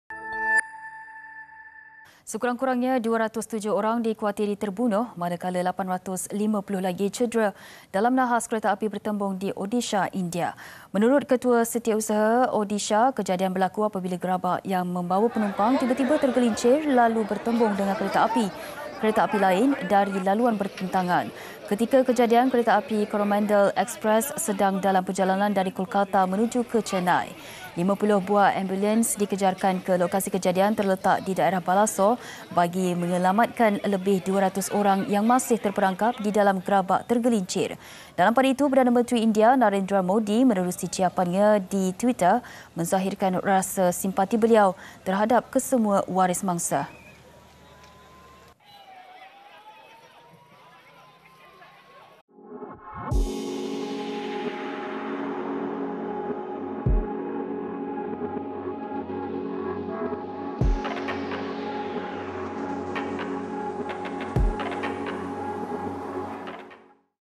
TRAGEDI | 207 Maut, 850 Cedera Kereta Api Bertembung
Sekurang-kurangnya 207 orang dikhuatiri terbunuh manakala 850 lagi cedera dalam nahas kereta api bertembung di Odisha, India.